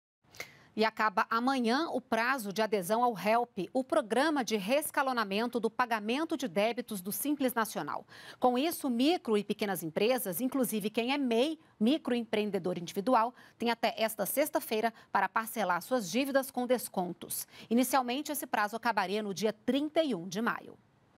Economia | Prazo de adesão ao Relp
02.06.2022 - Acaba amanhã o prazo de adesão ao Relp, o Programa de Reescalonamento do Pagamento de Débitos do Simples Nacional.